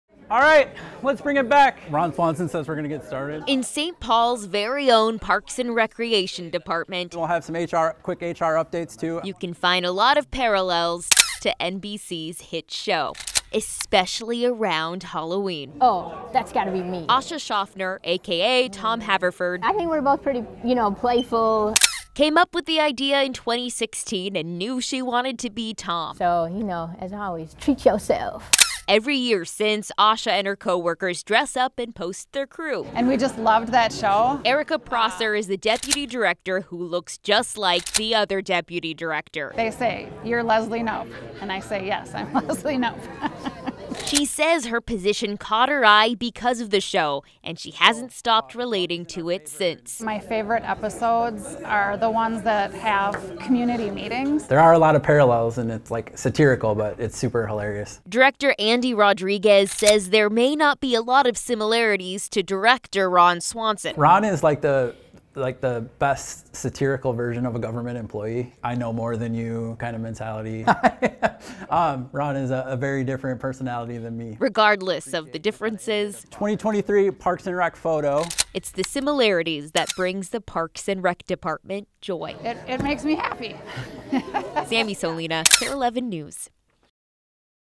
St. Paul Parks and Recreation dresses up like Parks and Recreation TV characters
Since 2016, the St. Paul Parks and Recreation department has used Halloween to dress like the cast from the hit TV show "Parks and Recreation." "I look forward to this every year!"